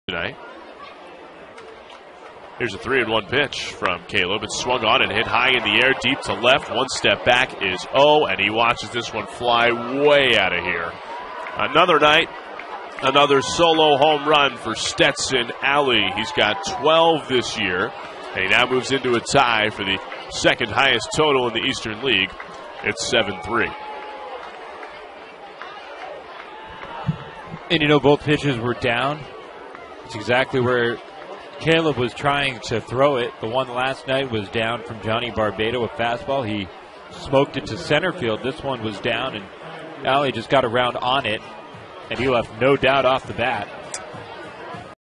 Curve's Allie cracks 12th homer
Altoona's Stetson Allie cracks his 12th homer in the Curve's 11-3 loss to Trenton